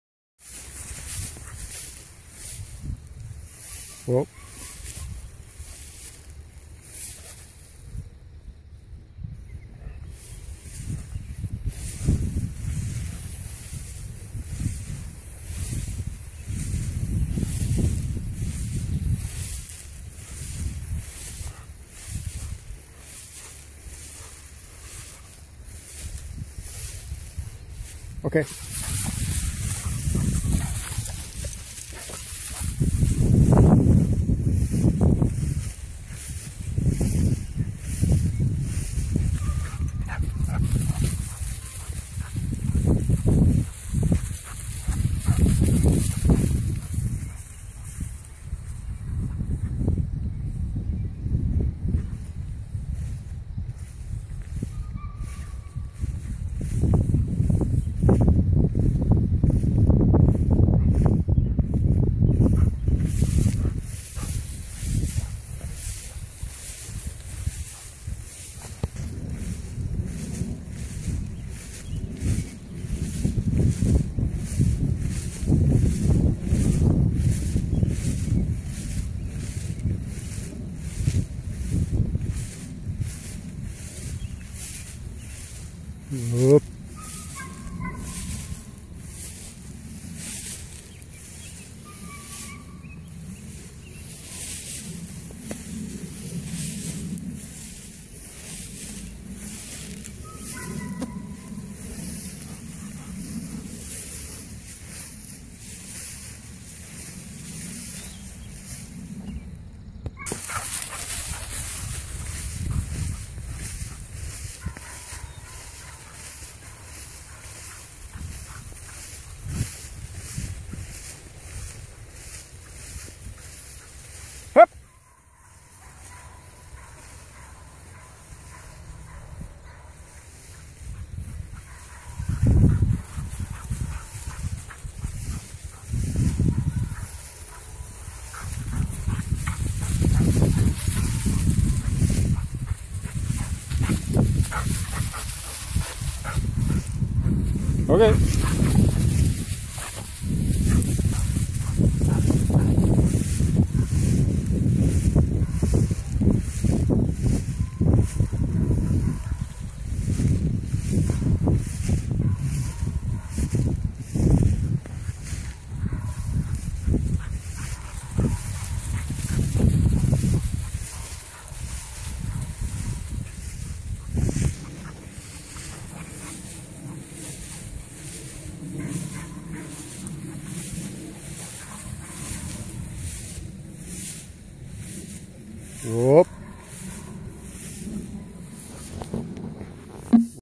Krecklaus Thunderstruck Angus v Riptraf a 10 month old German Shorthair pointer working birds.